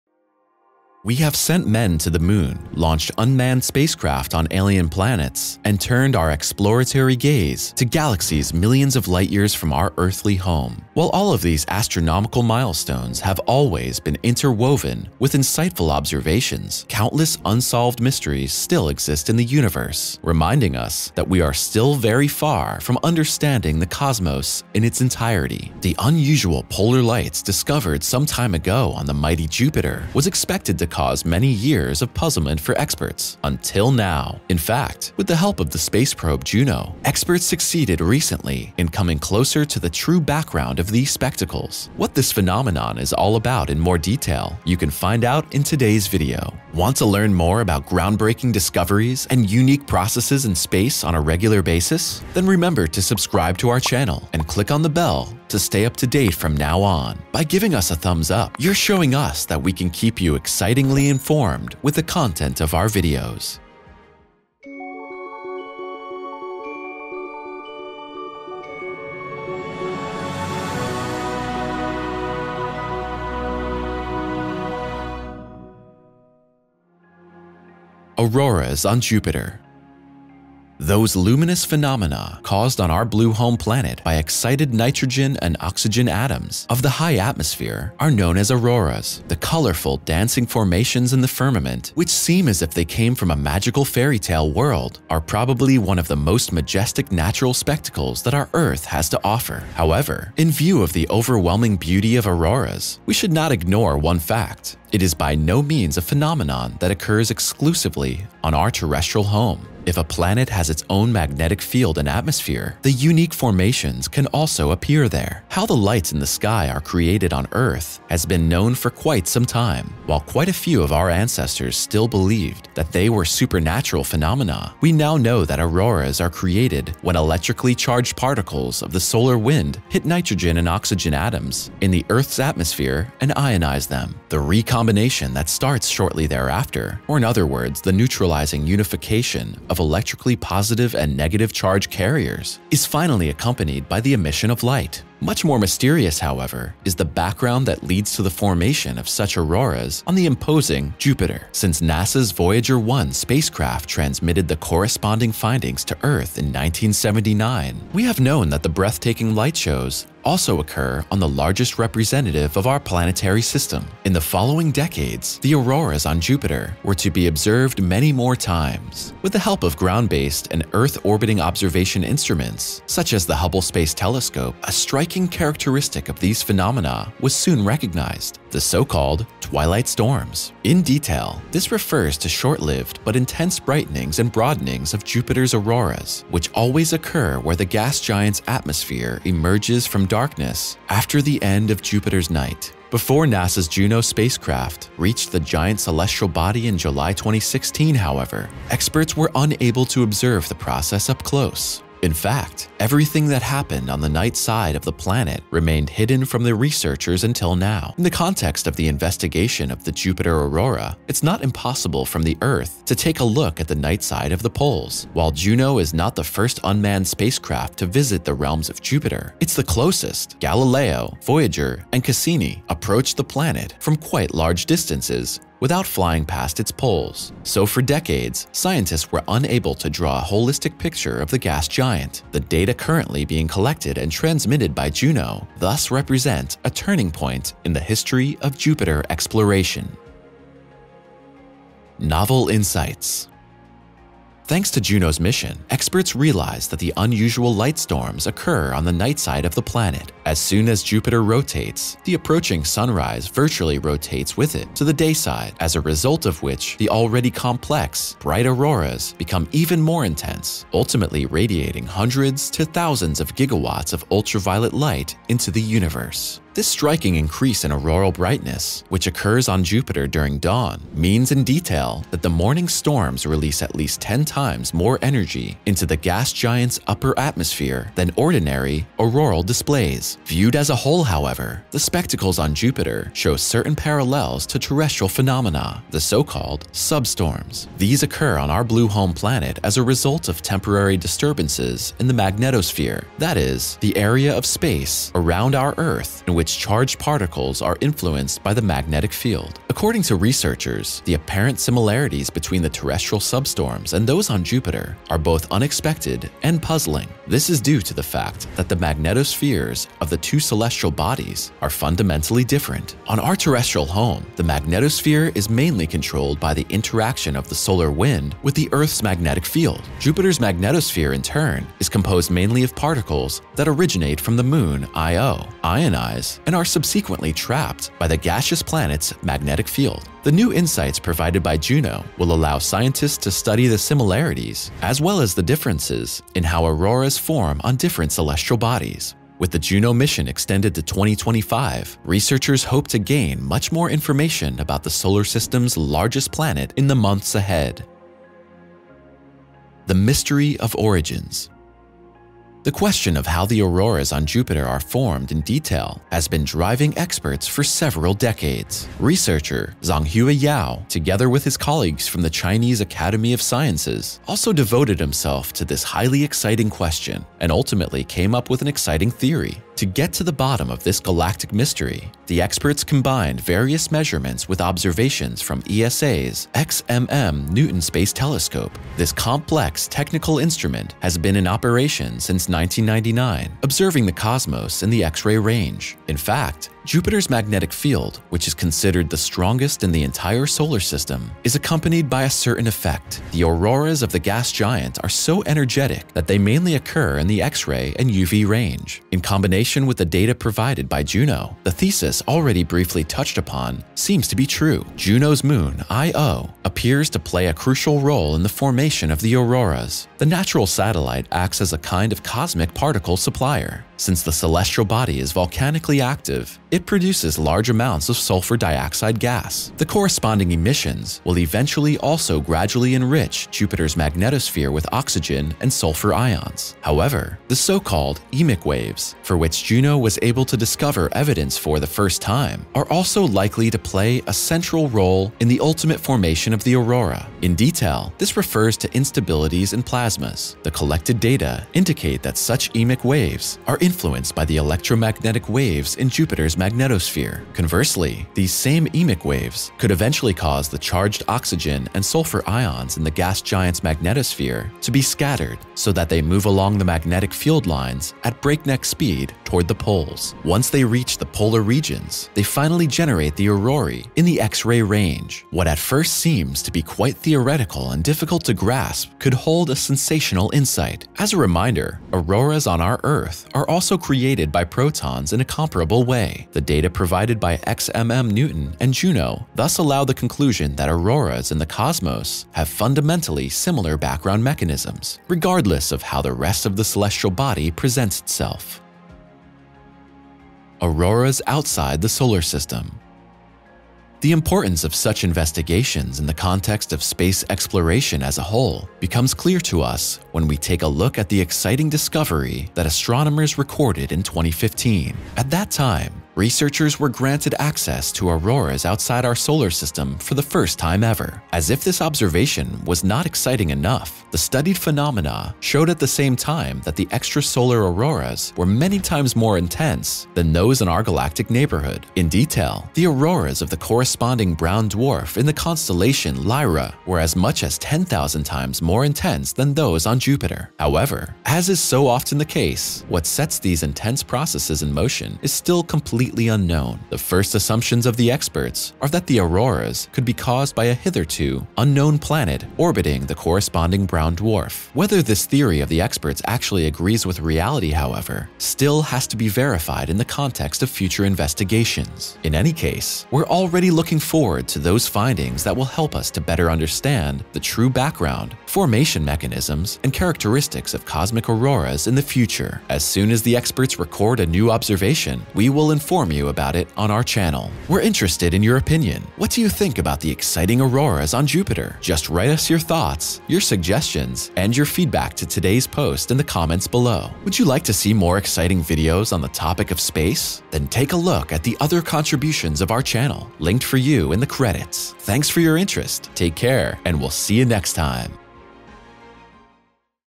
NASA Reveals the Dark Origins of Jupiter’s Lights!
We have sent men to the moon, launched unmanned spacecraft on alien planets, and turned our exploratory gaze to galaxies millions of light-years from our earthly home. While all of these astronomical milestones have always been interwoven with insightful observations, countless unsolved mysteries still exist in the universe, reminding us that we are still far from understanding the cosmos in its entirety. The unusual polar lights discovered some time ago on the mighty Jupiter was expected to cause many years of puzzlement for experts - until now! In fact, with the help of the space probe Juno, the experts succeeded recently in coming closer to the true background of these spectacles. What this phenomenon is all about in detail, you can find out in today's video!

Credit: NASA, ESA, ESO, SpaceX, Wikipedia, Shutterstock, ...